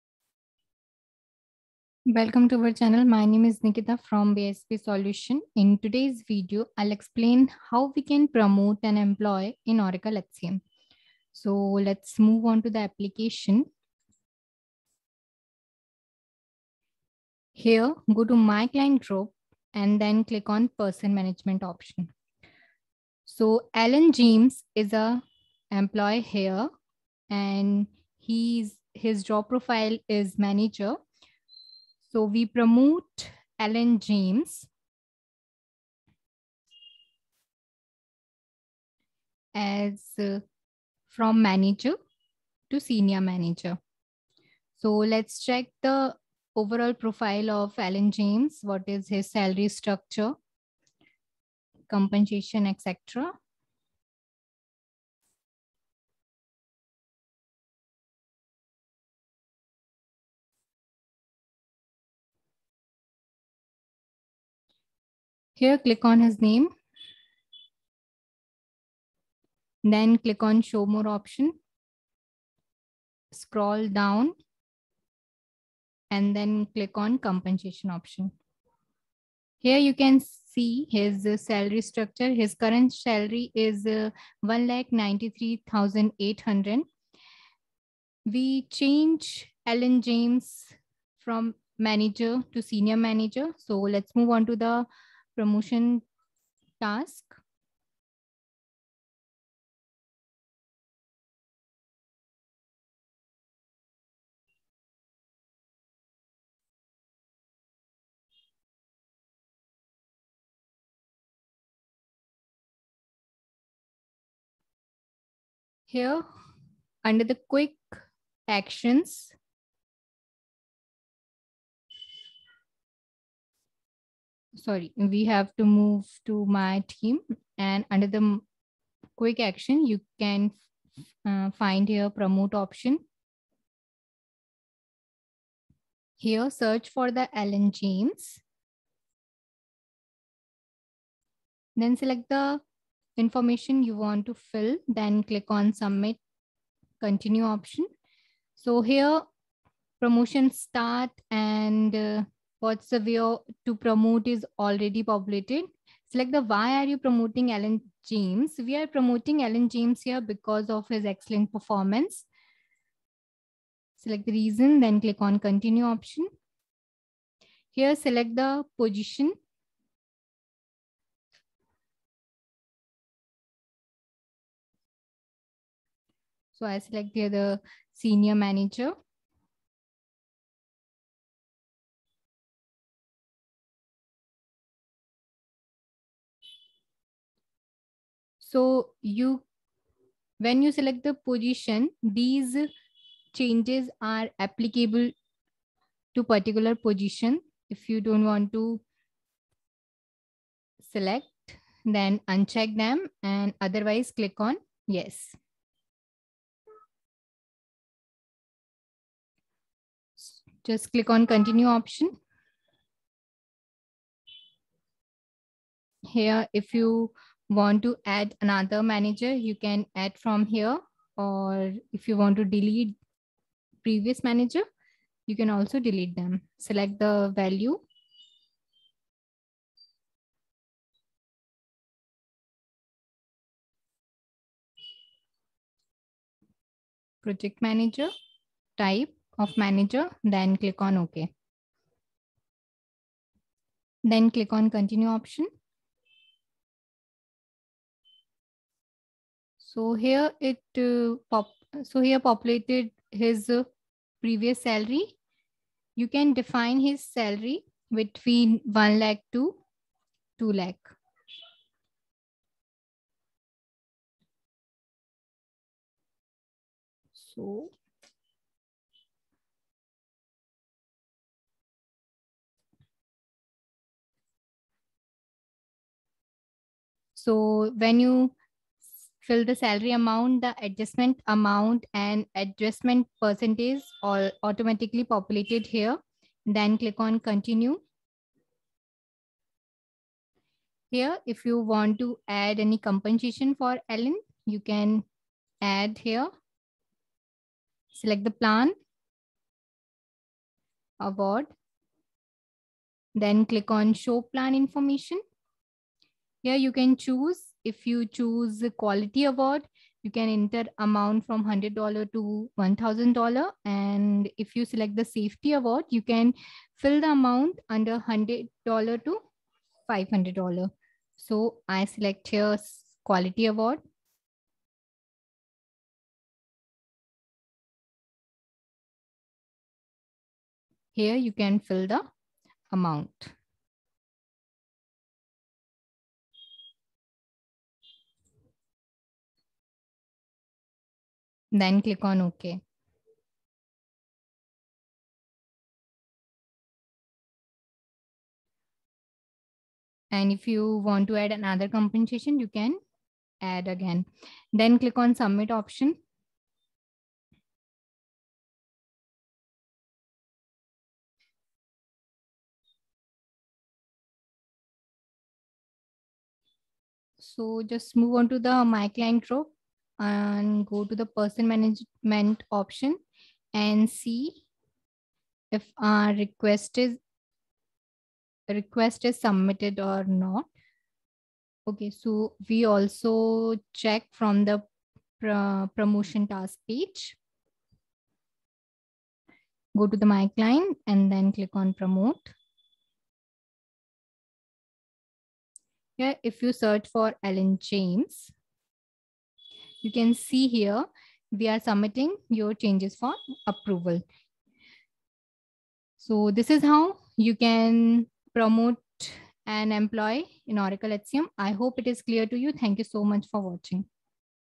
Oracle HCM Managing Promotion | Oracle Fusion HCM | Oracle HCM Training BISP
This video is all about Oracle HCM Managing Promotion.
Oracle HCM Promotion
How we can promote an employee in Oracle HCM: 00:00
Promote an employee in Oracle HCM with example: 00:14